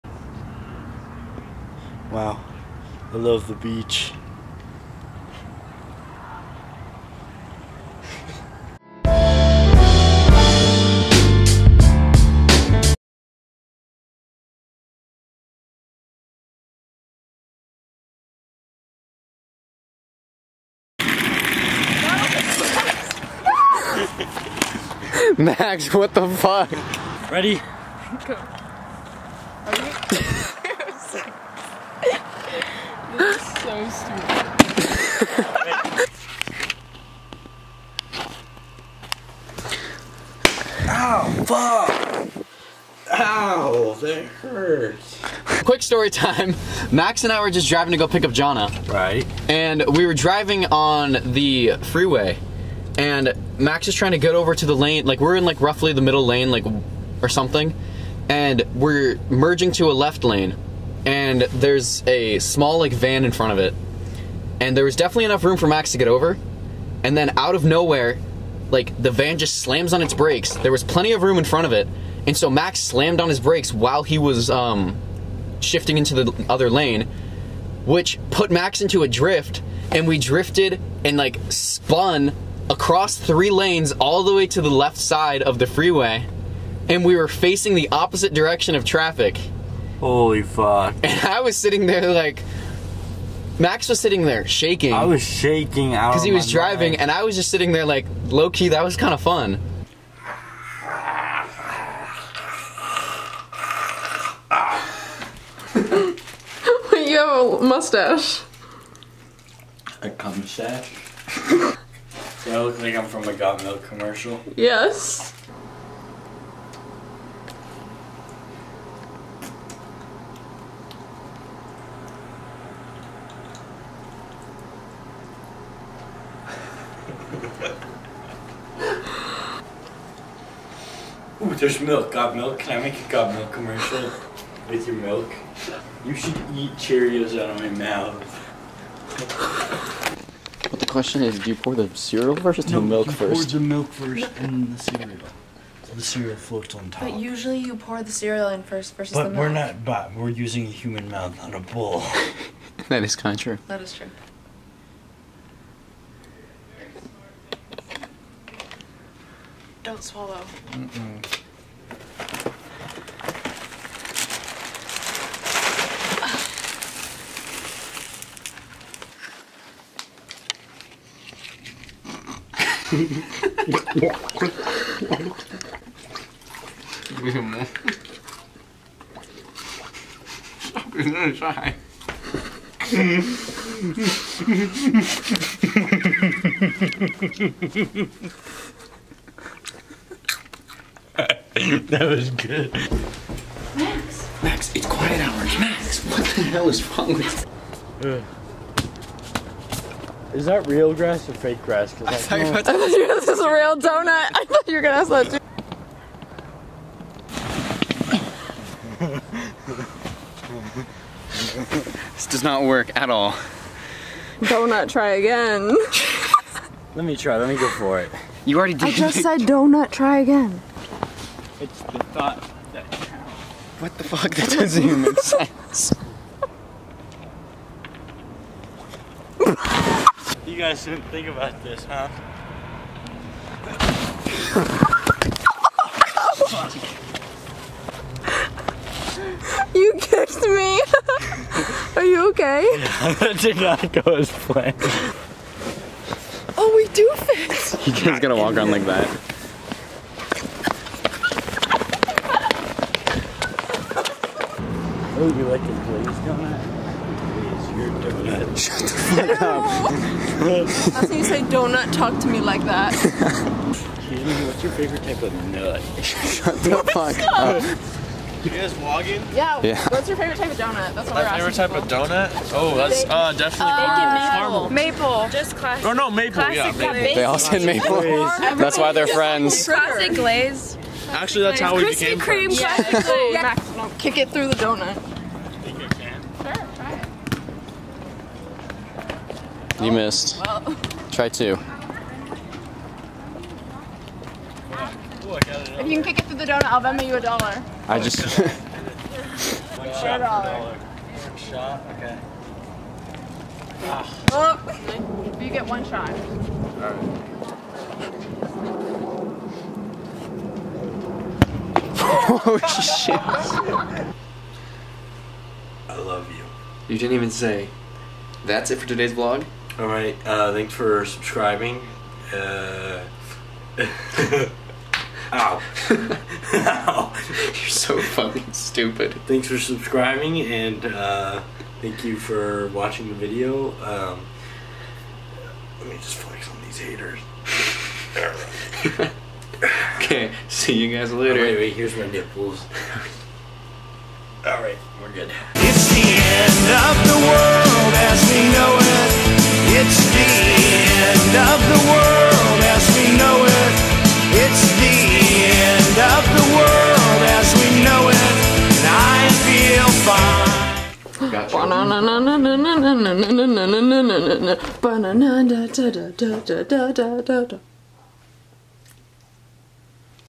Donut Themed Adventures (And I Eat Cereal Out Of Max's Mouth)
All of this footage is from prior to the suggested quarantine due to the COVID-19 outbreak, please stay safe and sanitary during this time!!

Follow Jadon on Instagram @jadontheasian